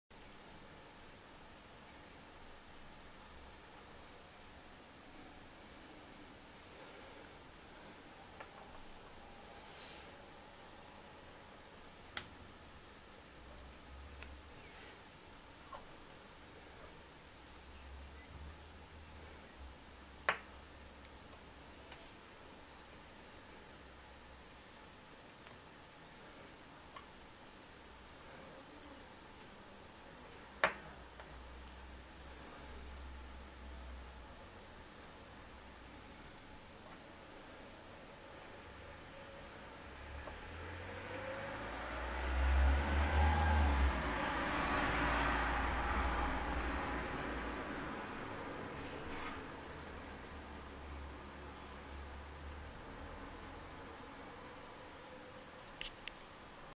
SMOKY QUARTZ FROM VALDEMANCO 20.MP4
SMOKY QUARTZ FROM VALDEMANCO, LOCATED IN A HYDROTHERMAL VEIN, 'LA CABRERA' PLUTONIC RANGE, MADRID-SPAIN

SOME LECTURES (SPANISH LANGUAGE):

(1) 'THE CHLORITES OF THE PEGMATITIC BODIES OF THE LA CABRERA PLUTON (SPANISH CENTRAL SYSTEM)' in

(2) 'FLUIDS ASSOCIATED TO EPIDOTE + (CHLORITE + QUARTZ) IN PEGMATITES FROM LA CABRERA PLUTON (SPANISH CENTRAL SYSTEM)' in

(3) ' Geocronología 2°'Pb/2°6Pb en cristal único de circón y Rb-Sr. ' in

CUARZO AHUMADO DE VALDEMANCO, EL PLUTÓN DE LA CABRERA

GEOLOGY GEOLOGIA MINERALS MINERALES QUARTZ CUARZO